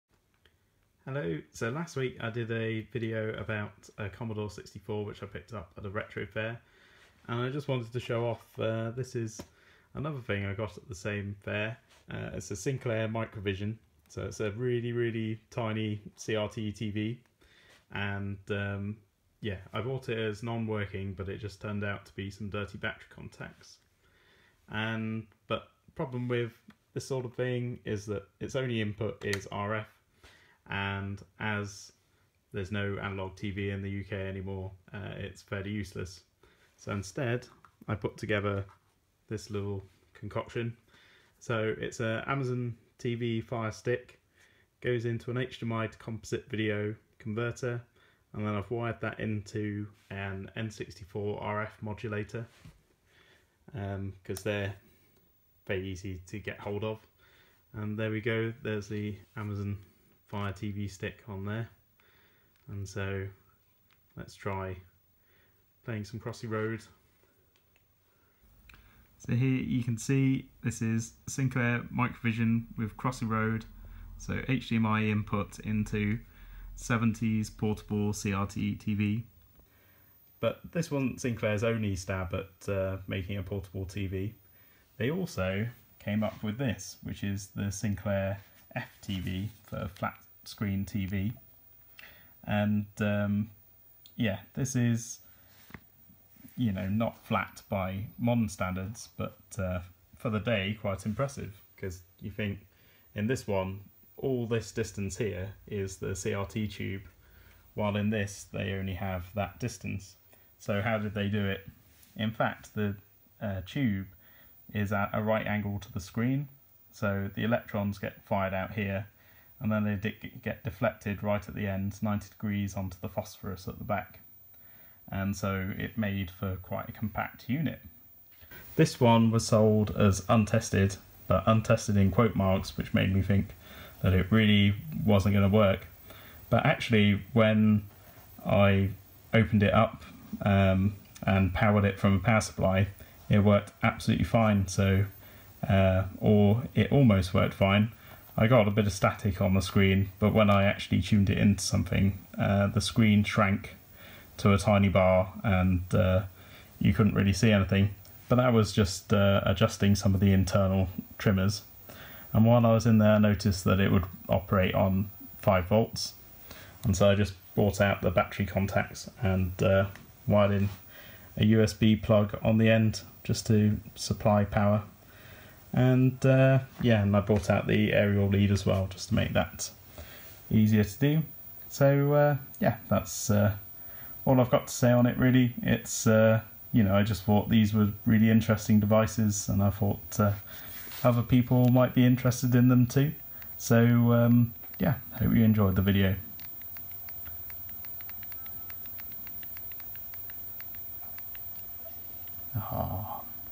HDMI into Sinclair Microvision + TV80 (Flat Screen Pocket TV)
Quick video about two curious devices I've picked up recently.

Using a N64 RF modulator and a HDMI to composite adapter I managed to get these ancient CRTs to connect to modern inputs